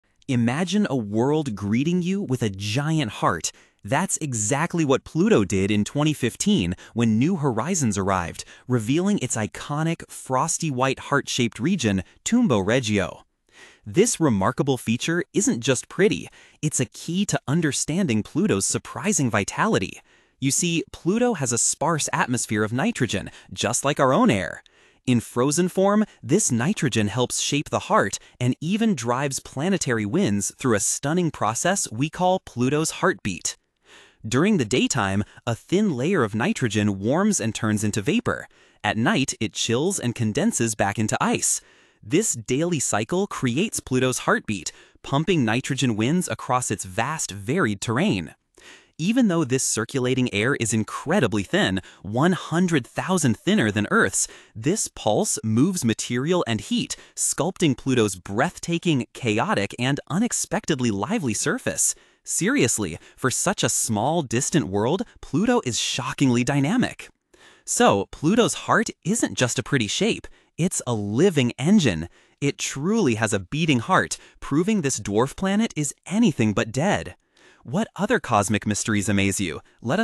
Pluto's Beating Heart: How a Dwarf Planet Thrives
Beyond its icy reputation, Pluto harbors astonishing secrets! What if this distant dwarf planet isn't just a frozen relic, but a world with a surprising "beating heart"? In this video, we embark on an incredible journey to uncover how Pluto truly thrives. Prepare to be captivated as we explore its vast, dynamic nitrogen ice plains, towering water-ice mountains, and even the tantalizing possibility of a subsurface ocean. Discover the powerful geological forces shaping its surface and the mysterious ways its thin atmosphere behaves. Pluto is far more active and complex than anyone ever imagined. Join us to unravel the vibrant, enigmatic life of this small but mighty world, proving that even at the solar system's edge, planets can pulsate with vitality.